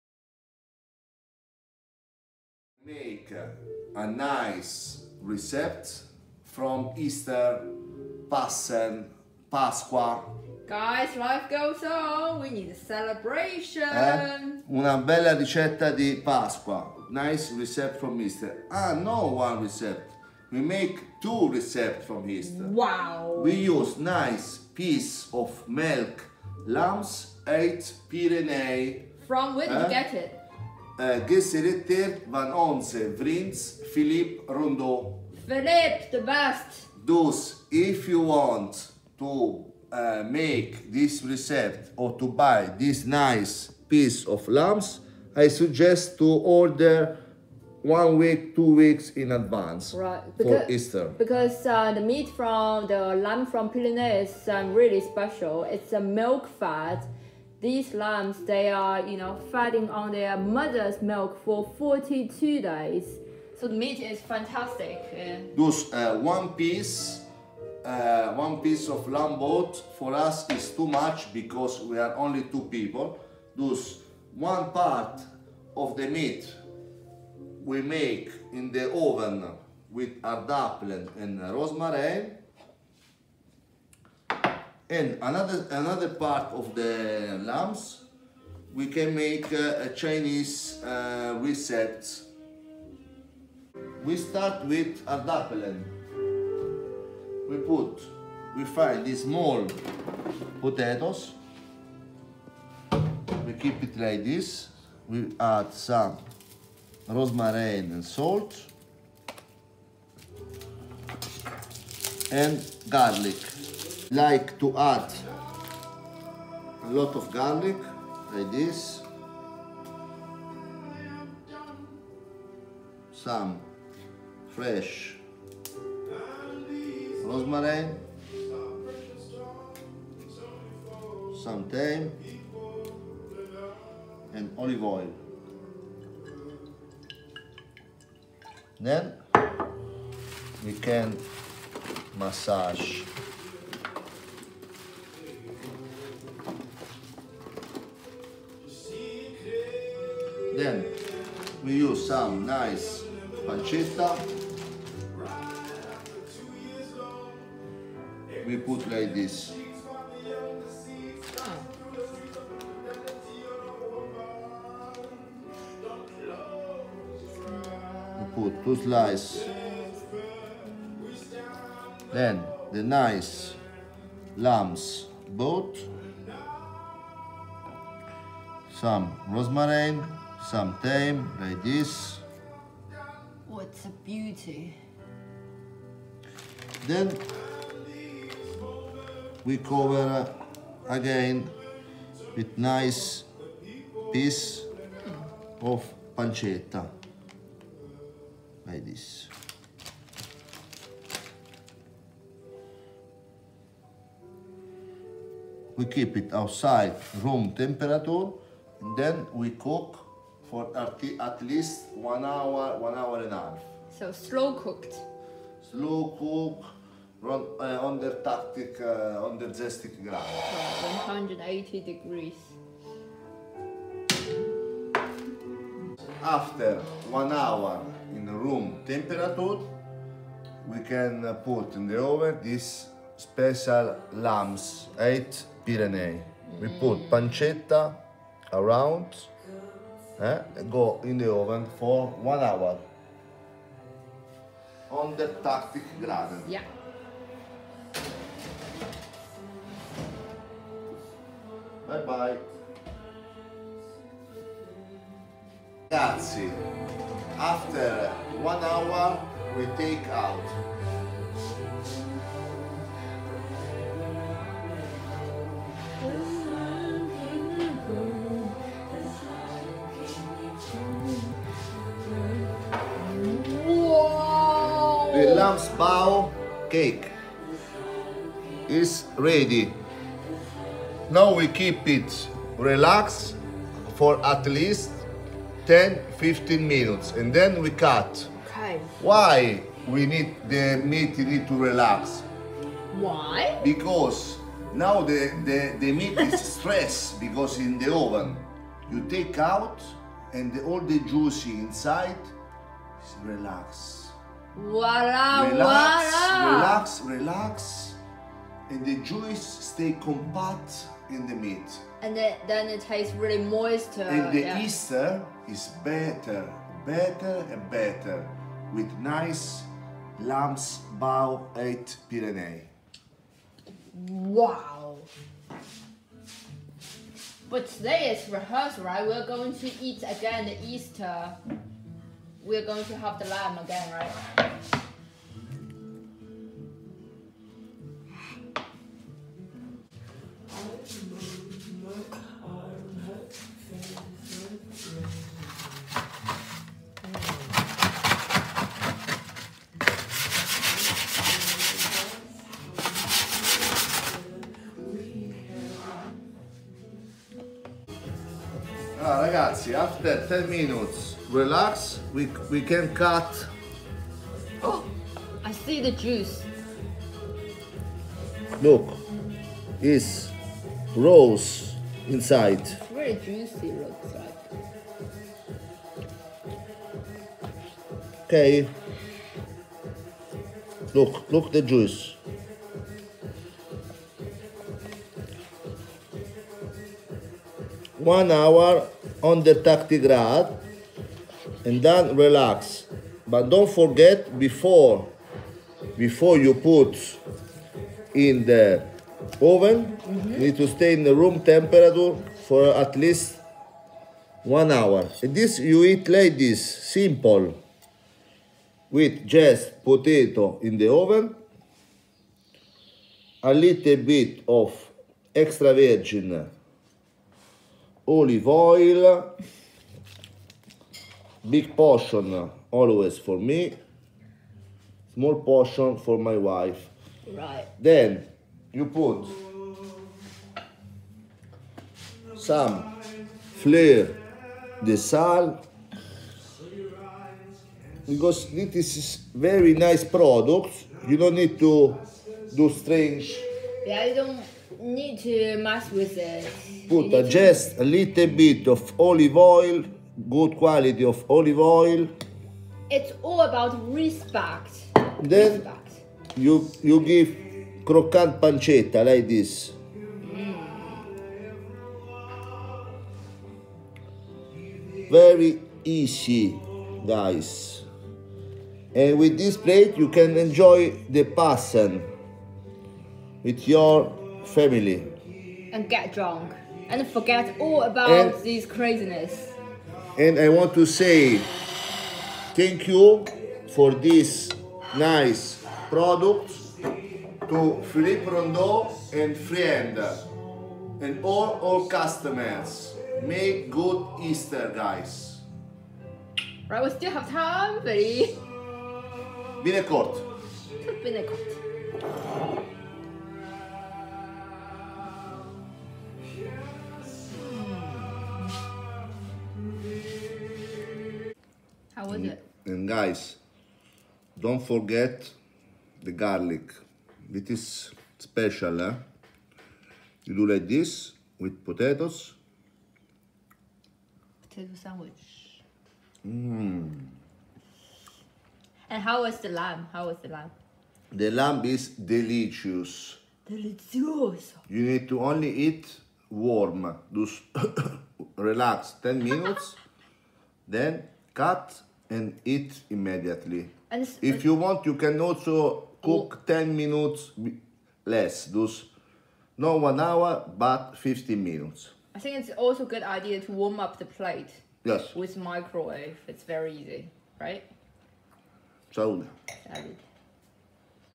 How to cook roast Pyrenees Milk Fed Lamb? Stay At Home With Rossi Day 16 (Easter special recipe 1)
Are you one of the many who are locked down at your home because of Covid-19 virus outbreak? Stay at home, stay cool and cook along with Felice Miluzzi, Chef and owner of Ristorante Rossi & Rossi Trattoria+Atelier, Leuven Belgium